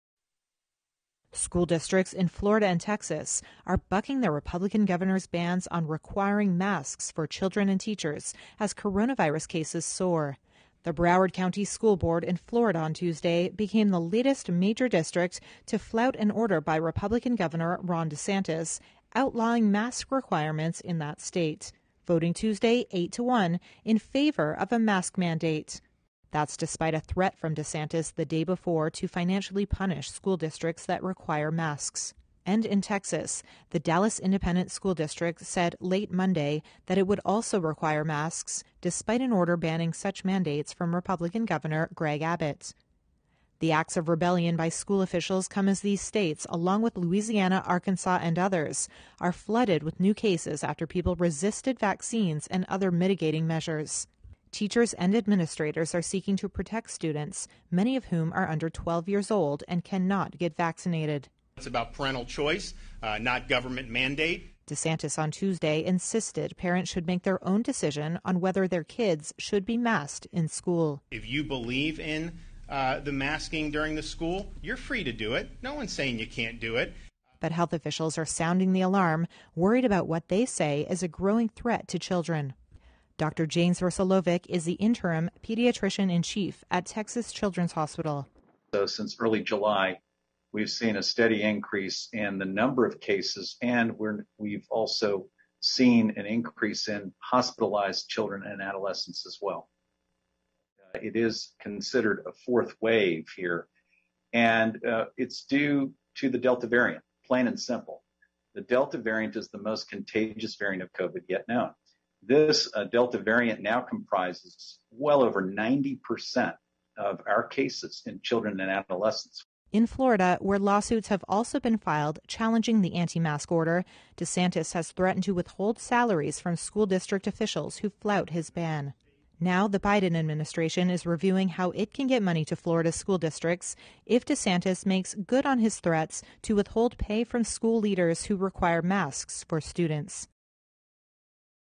Florida,Texas schools defy governors’ bans on mask mandates as COVID cases soar
School districts in Florida and Texas are bucking their Republican governors' bans on requiring masks for children and teachers as coronavirus cases soar in areas with low vaccination rates